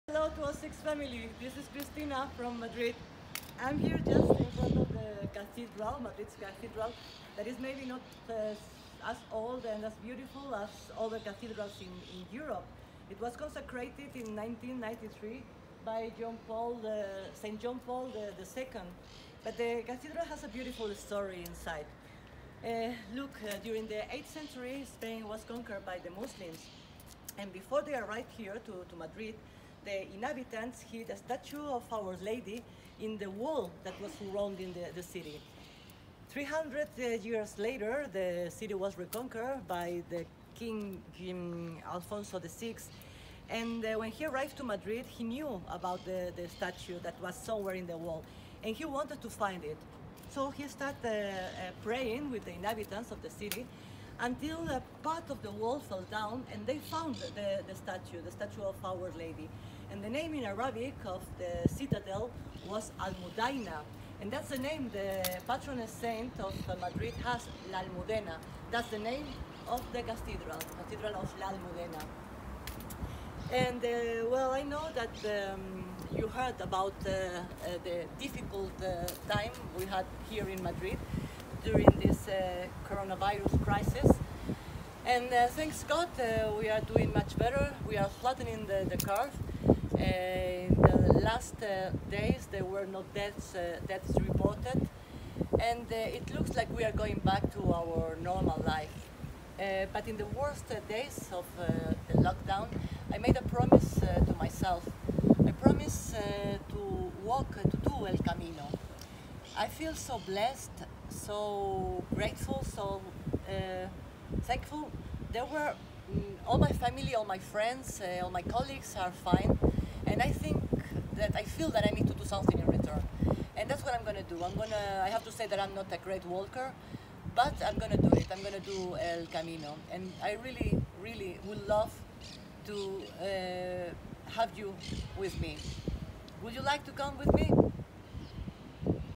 Checking in on Madrid with our Guide Christina
Our guide Christina opens up about how Madrid is recouping from the pandemic and tells us about a promise she made to herself while quarantining, to walk the El Camino trail in thanksgiving to God for keeping her family, friends, and colleagues safe.